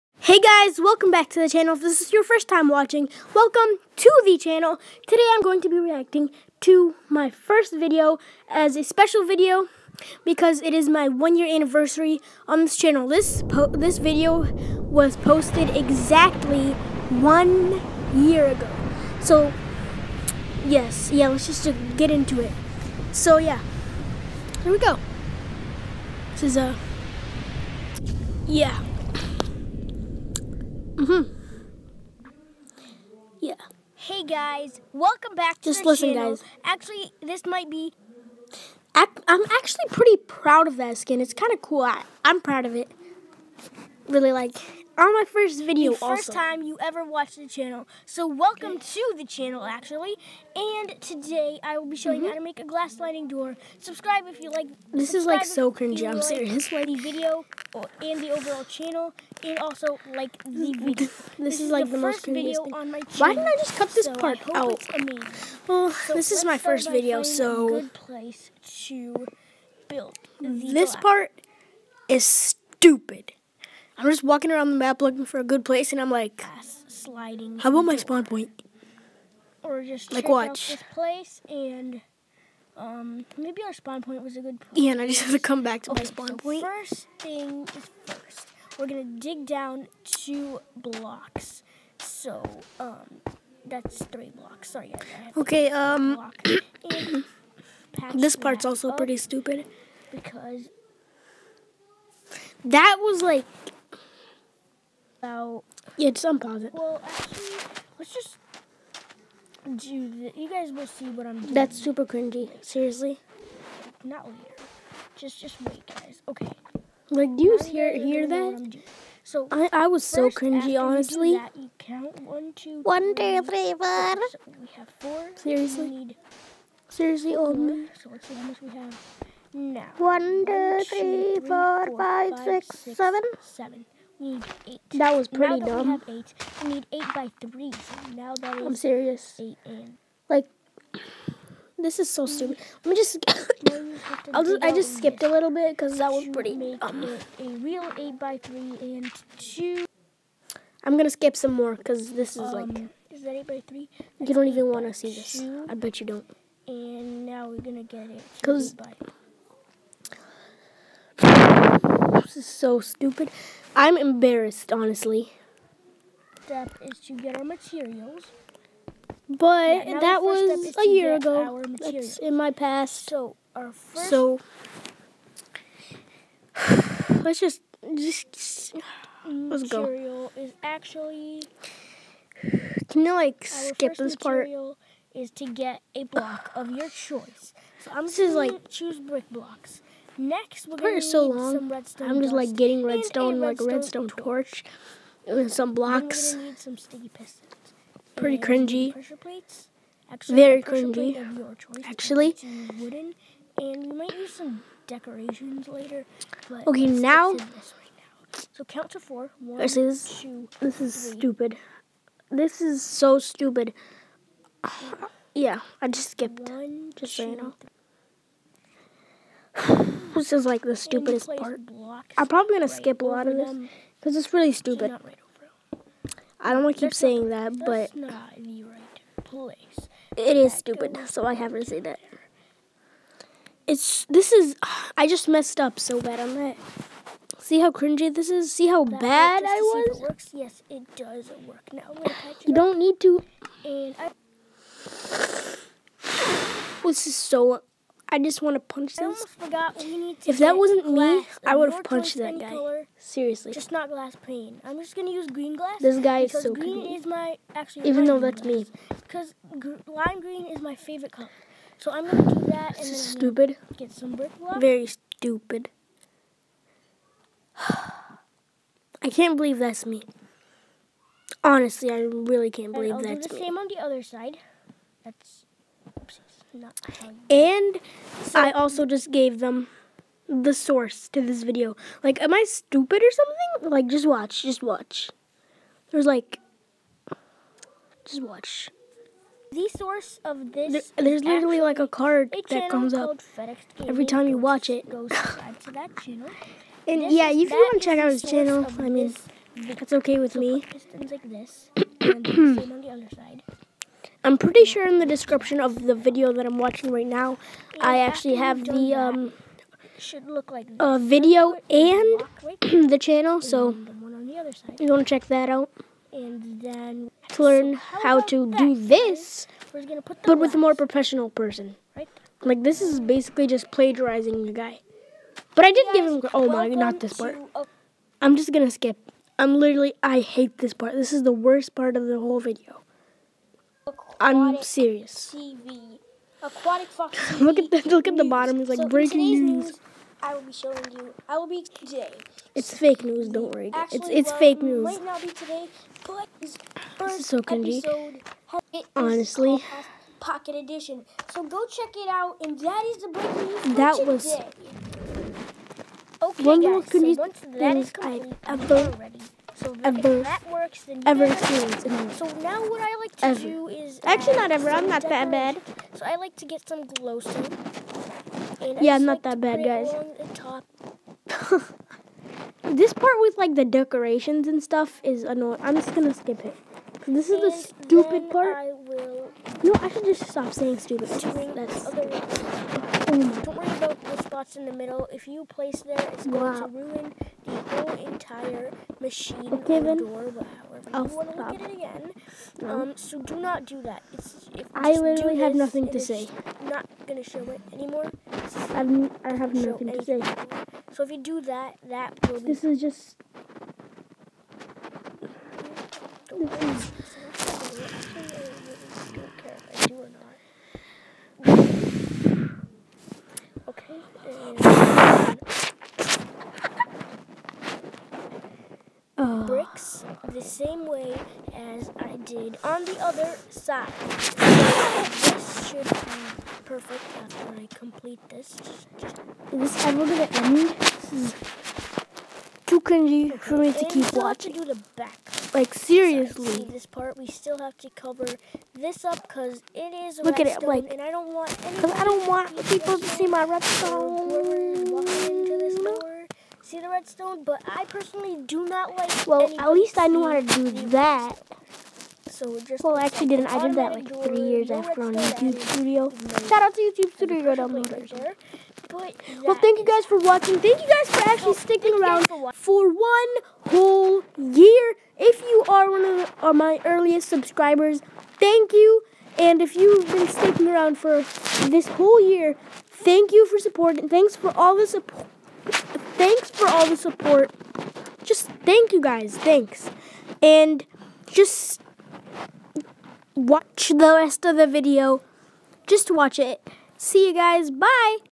1 Year Anniversary!! Reacting To My “Fur-st” Video!
This is my 1 year anniversary! Thx for sticking around all year!